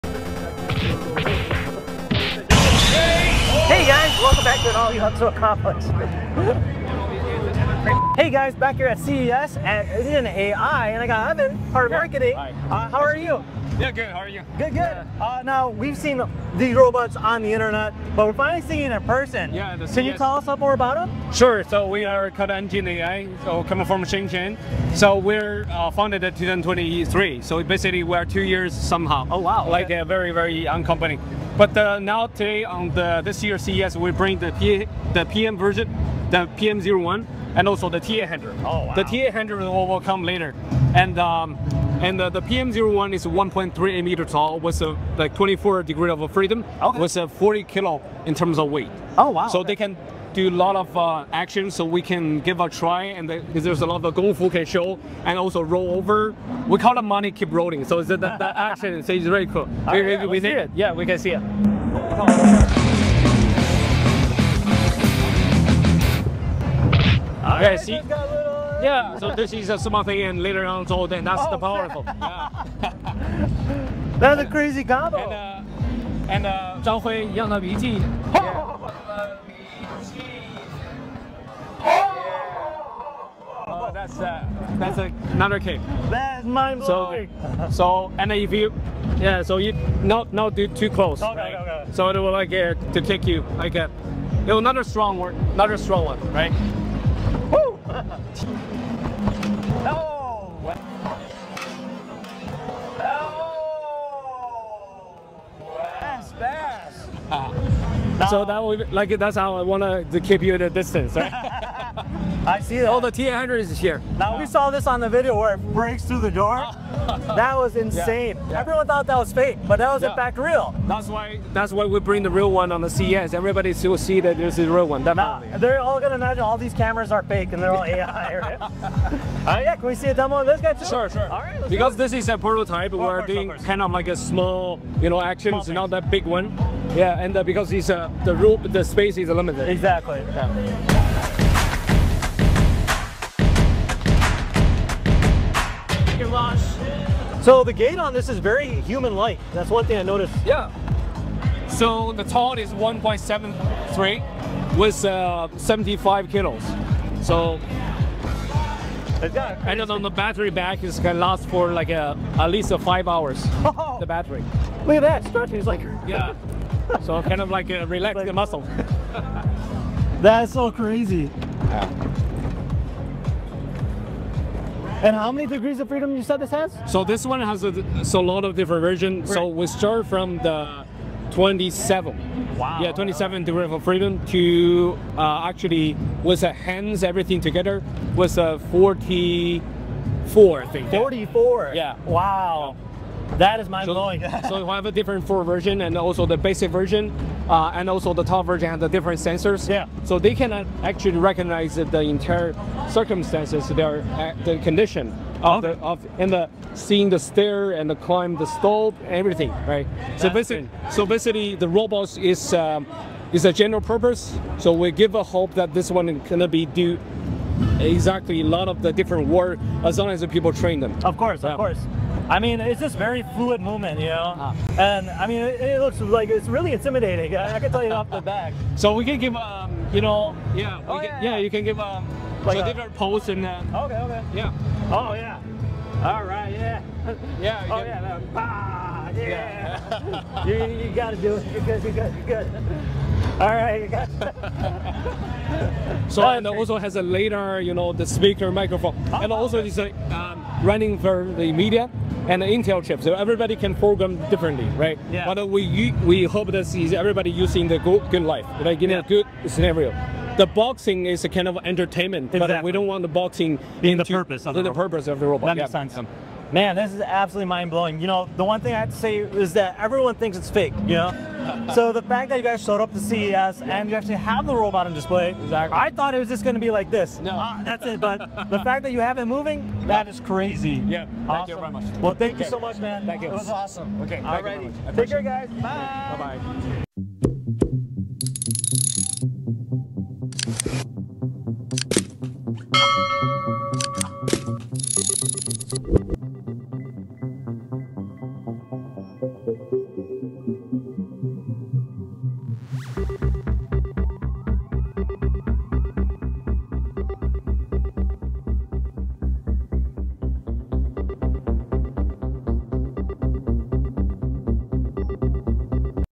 Ultimate Fighting Humanoid Robot T800! EngineAI | CES 2026
This was my most favorite robot at CES! Should we buy one? 🤔

At CES 2026, I got a close look at one of the most talked-about humanoid robots on the show floor — the EngineAI T800. Also we check out the PM01

00:00 Intro
00:42 PM01 showcase
02:20 T800 showcase